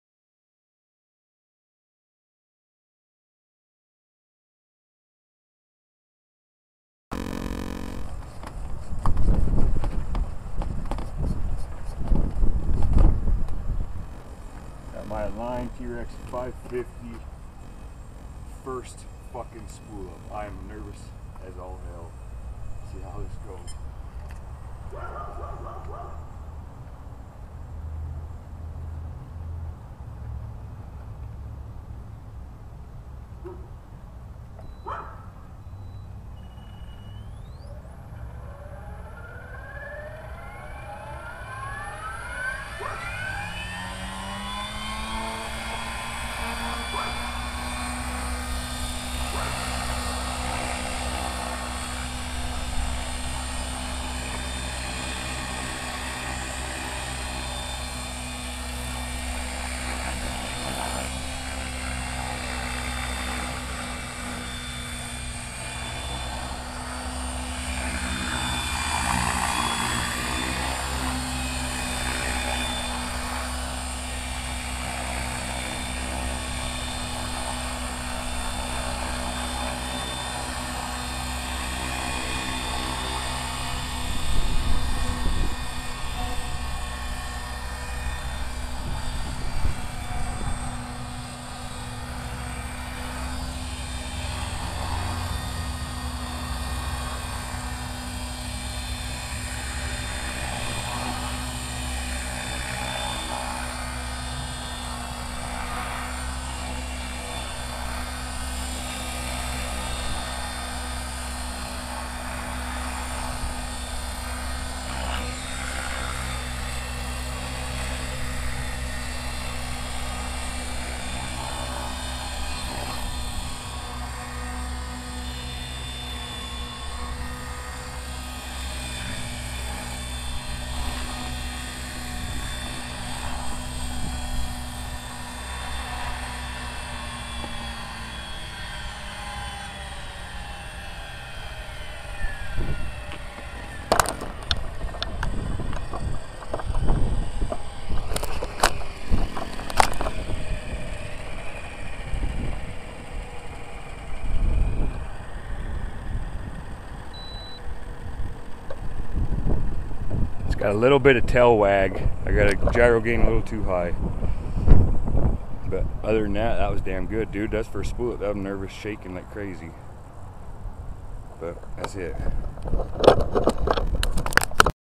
Align Trex 550e Pro DFC first spool up
the first flight after the build and set up of my brand new align trex550e Pro DVC Helicopter,     the tail gain is a little high causing the tail to  wag but I have since dialed it down eliminating it completely.